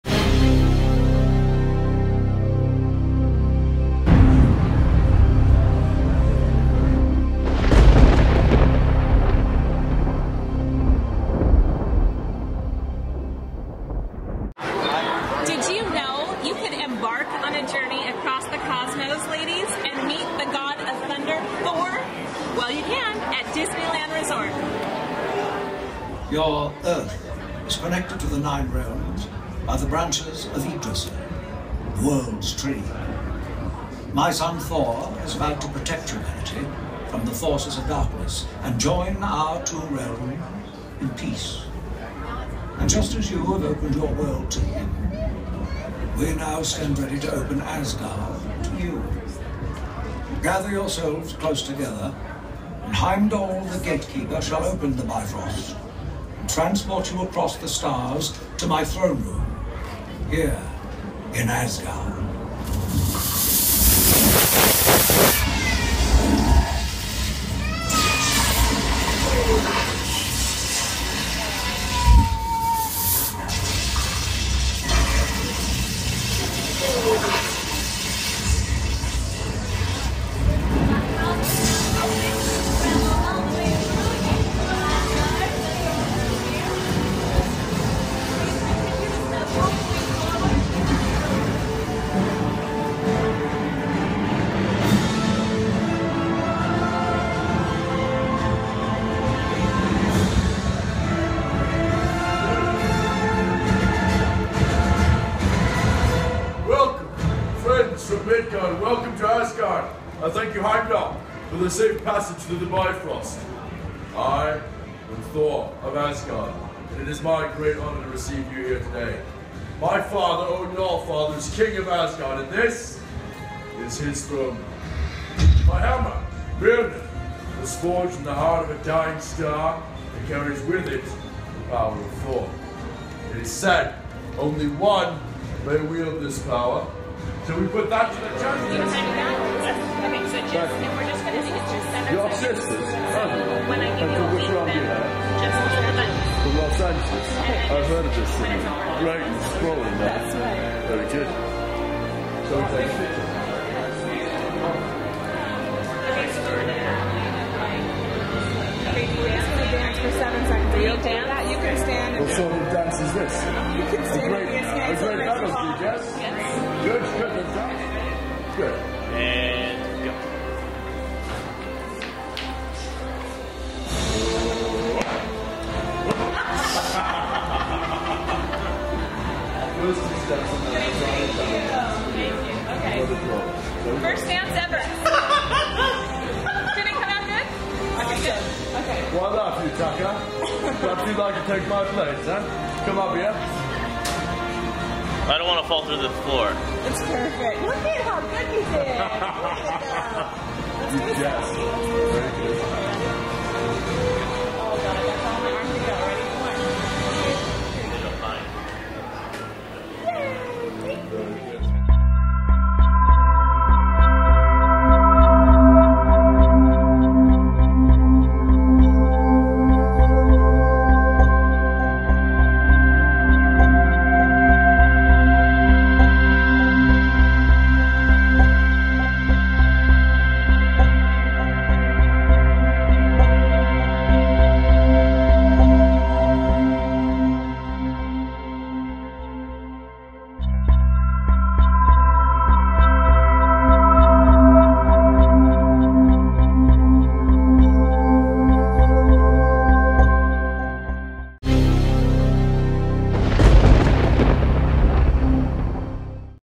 THOR Meet and Greet at Disneyland
for more happenings at the Disneyland Resort!

Now at Innoventions at Disneyland in Tomorrowland is the Thor: Treasures of Asgard Experience. The Bifrost has opened between Earth and Asgard—and you're invited to come explore a world of gods. Embark on a journey across this "cosmic bridge" connecting Innoventions at Disneyland park with the home world of Thor, the Mighty Avenger—and star of the film Thor: The Dark World.